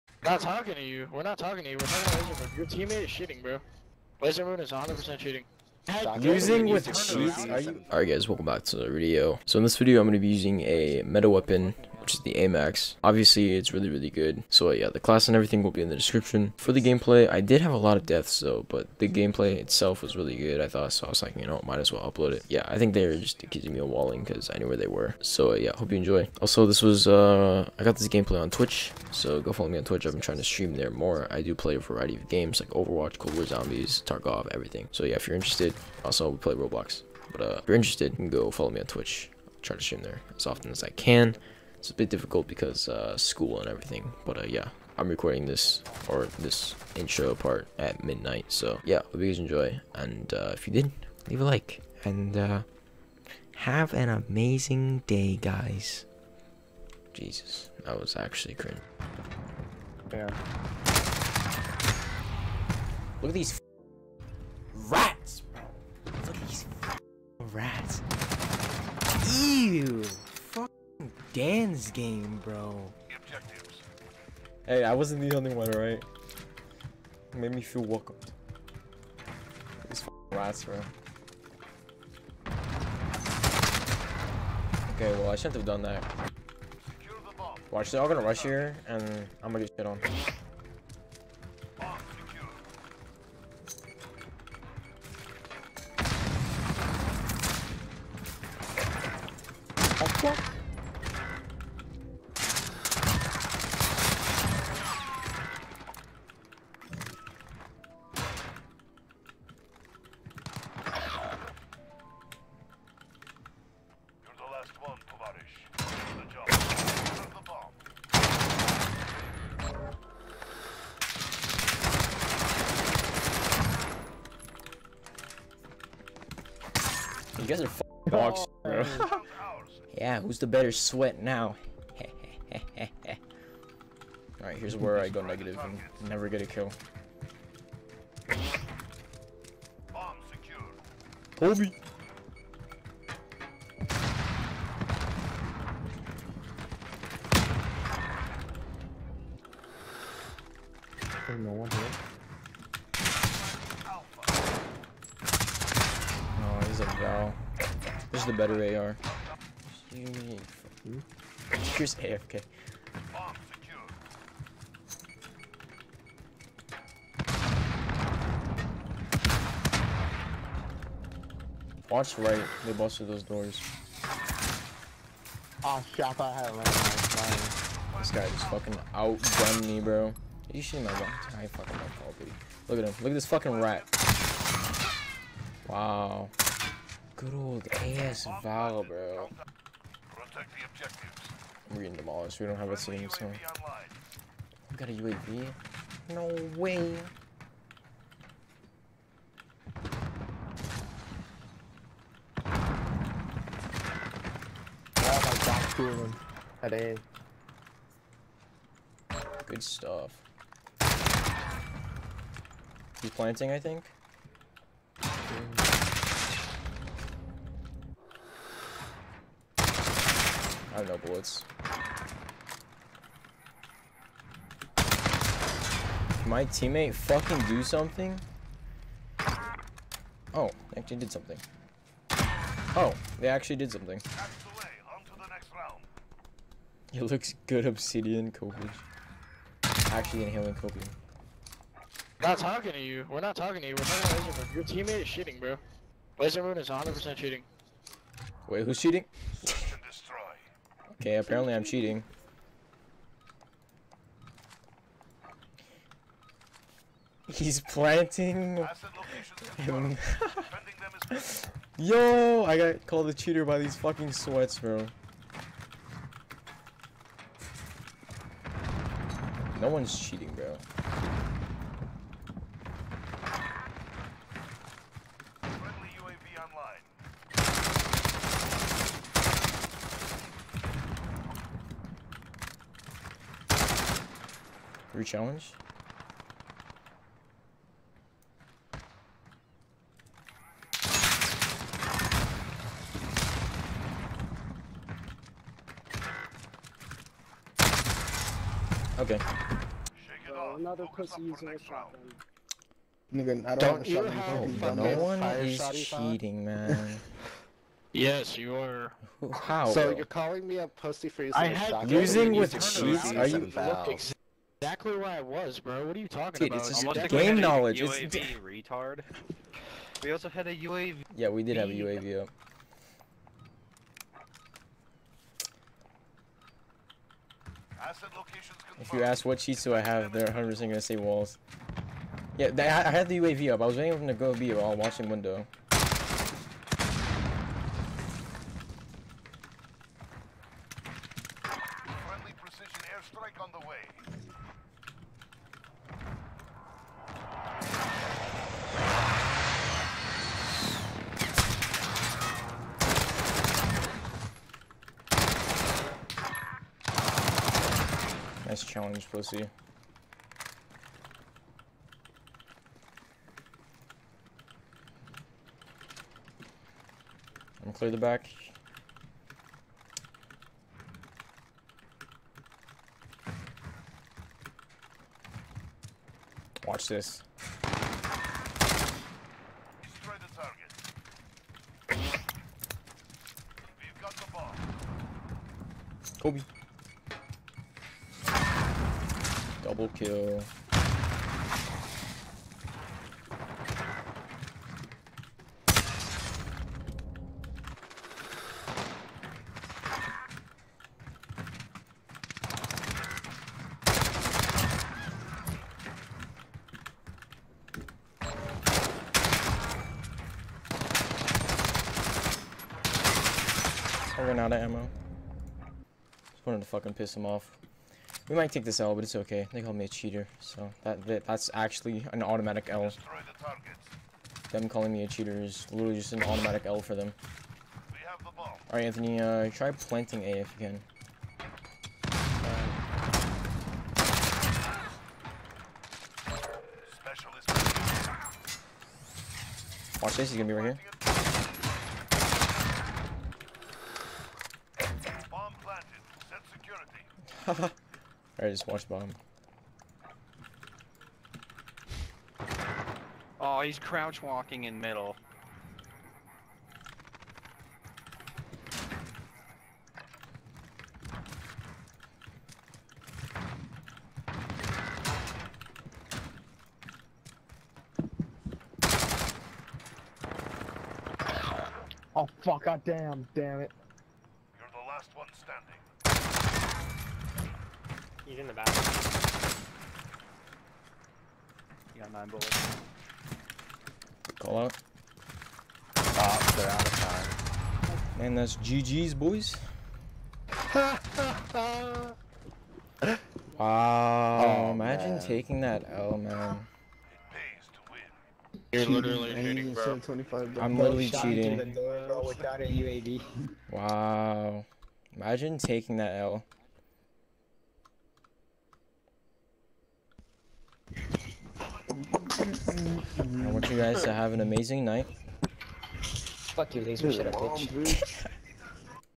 i used a meta weapon and got accused of cheating immediately...
ayo leave a like for free candy

Compensator (Muzzle)
XRK Zodiac S440 (Barrel)
No Stock (Stock)
Commando Foregrip (Underbarrel)
XRK CR-56 Stippled Wrap (Rear Grip)